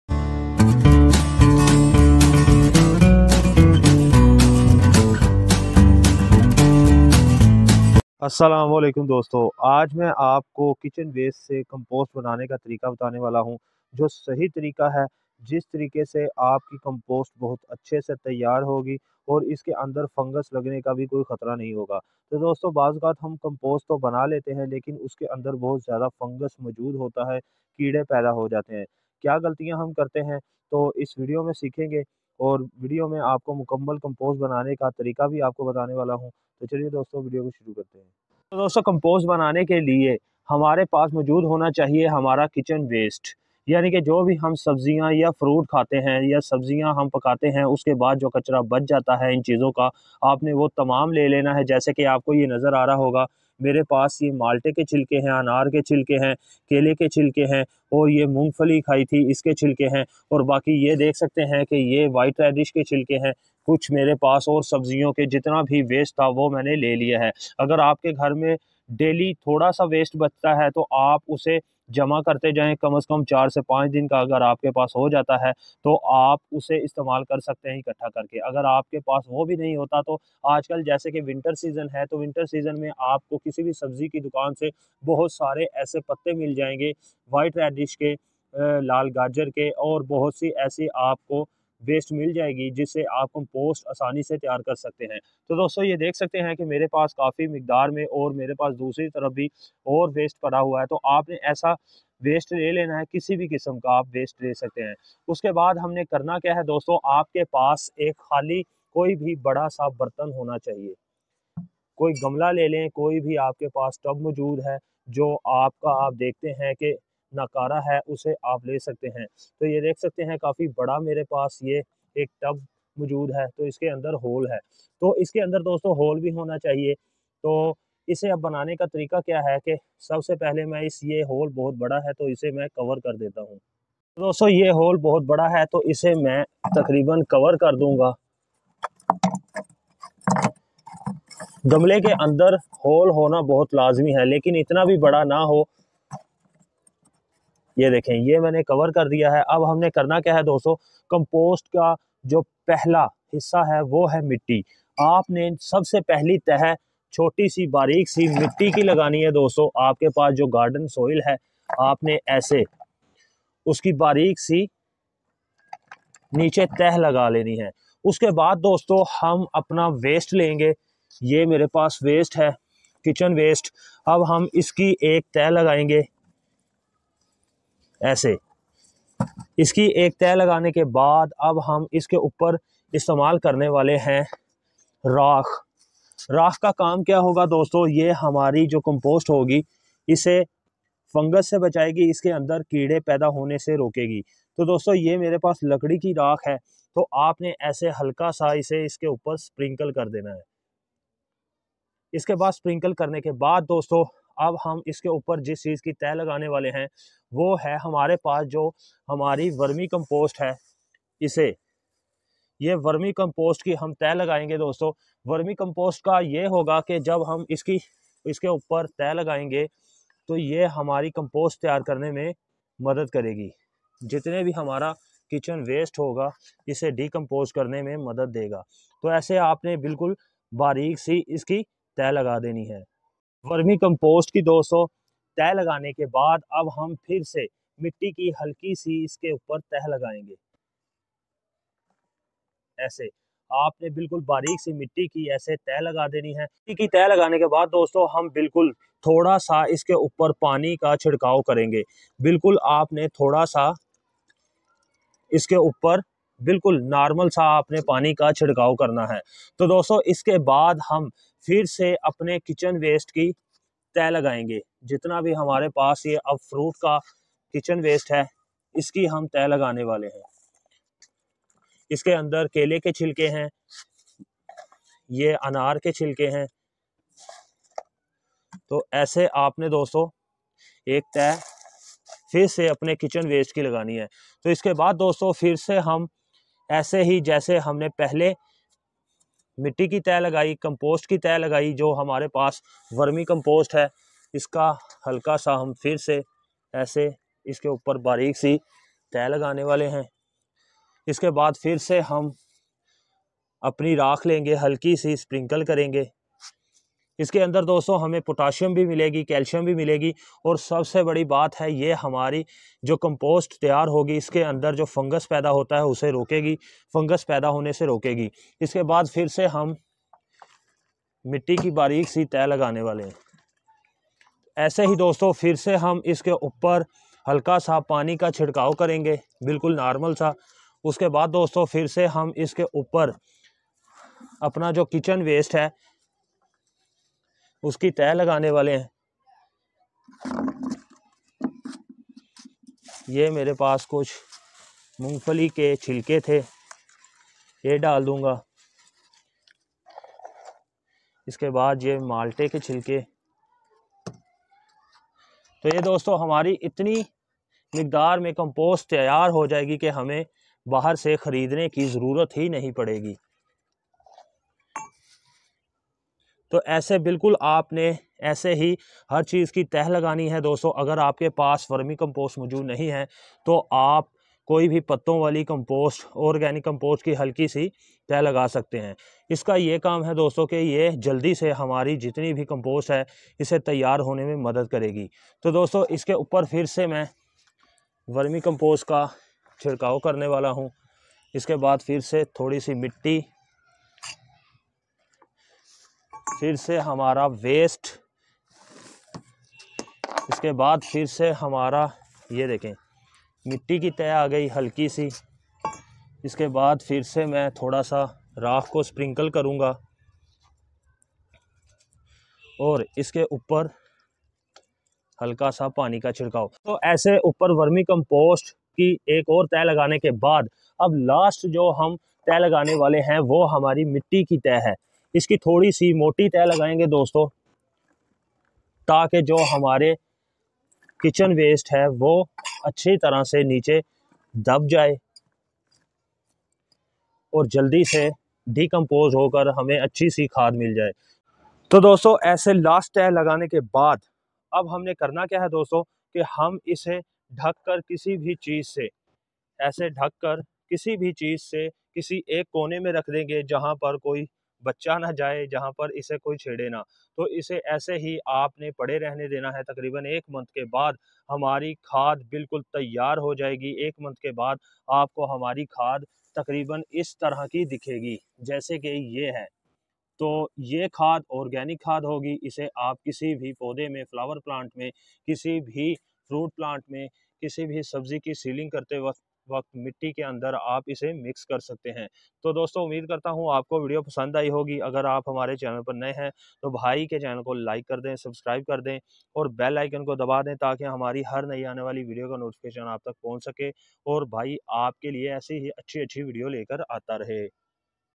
How to make kitchen waste compost at home | کیچن کے کچرے سے کھاد بنانے کا طریقہ ۔
How to make kitchen waste compost at home easy to way fungus free compost